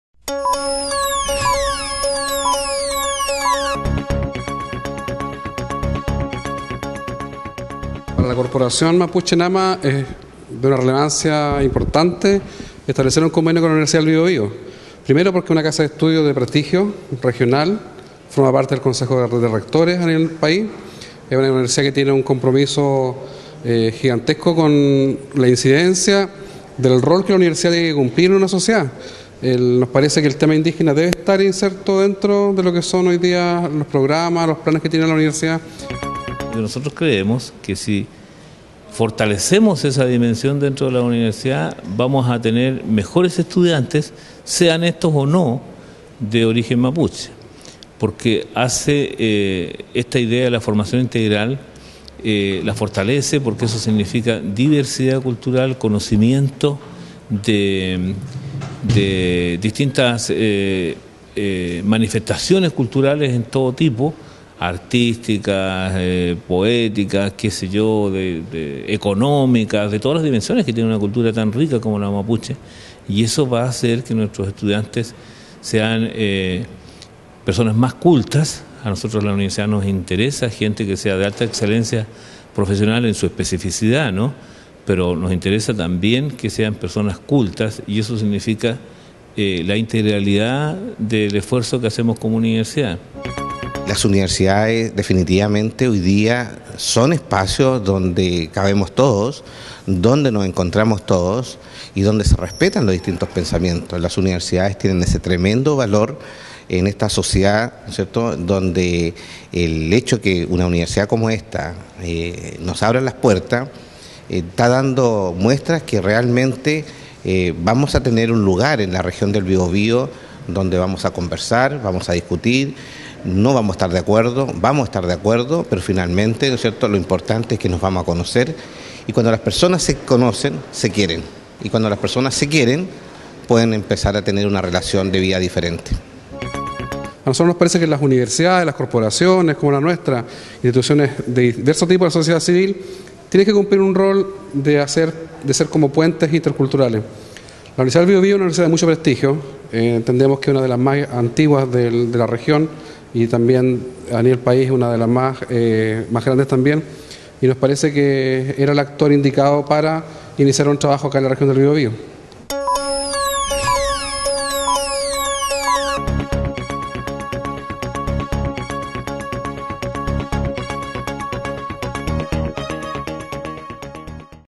Convenio con corporación mapuche contribuye a enriquecer formación de estudiantes UBB
La satisfacción de contribuir a la comprensión y promoción de la cultura mapuche y, con ello, a la construcción de un Chile multicultural, compartieron el rector de la UBB, Héctor Gaete Feres, y el presidente de la Corporación de Desarrollo Integral Mapuche Enama, Hugo Alcamán Riffo, al firmar un convenio marco de colaboración entre ambas corporaciones. De esta forma -coincidieron-, queremos aportar a la respetuosa convivencia intercultural y a la paz social en nuestro país.